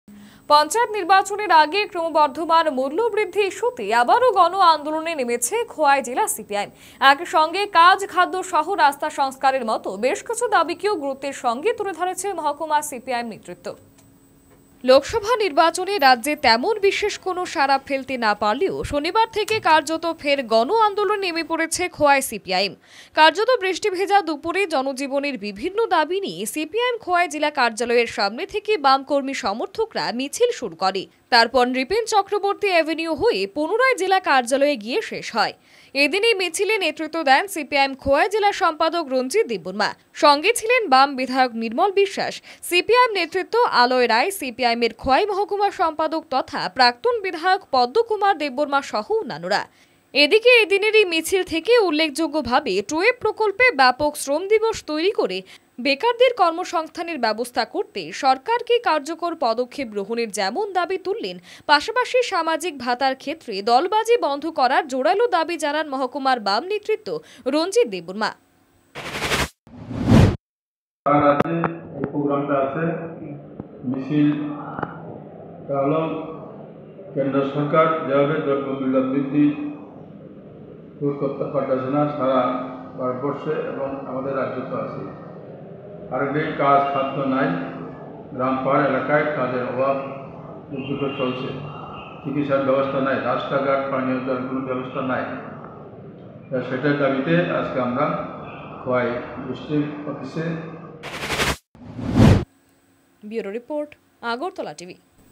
পঞ্চায়েত নির্বাচনের আগে ক্রমবর্ধমান মূল্যবৃদ্ধি ইস্যুতে গণ আন্দোলনে নেমেছে খোয়াই জেলা সিপিআইএম
পঞ্চায়েত  নির্বাচনের আগে ক্রমবর্ধমান মূল্যবৃদ্ধি ইস্যুতে আবারো গণ আন্দোলনে নেমেছে খোয়াই জেলা সিপিআইএম |  একই সঙ্গে কাজ খাদ্য সহ রাস্তা সংস্কারের মত বেশ কিছু দাবিকে ও গুরুত্বের সঙ্গে তুলে ধরেছে  মহকুমা সিপিআইএম নেতৃত্ব |